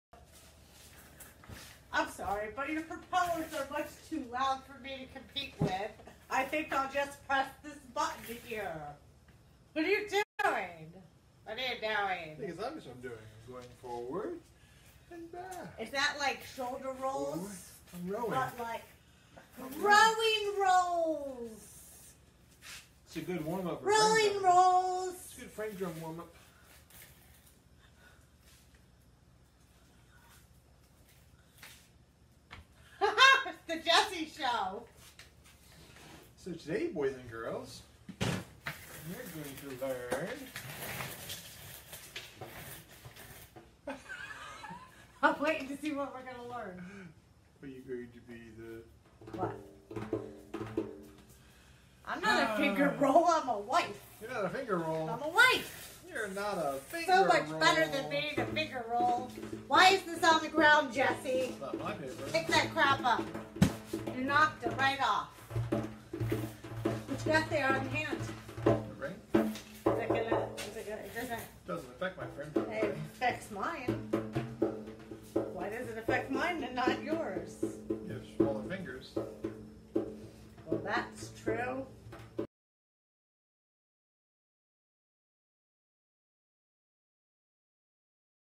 I am not a finger roll I am your wife
Seaweed and I have a lot of fun together just horsing around; it is a very important and special part of our relationship with each other.   So when we capture those special moments that are better than the average fun we have we like to share them because hell they are pretty funny sometimes.  I hope you enjoy our antics as much as we do.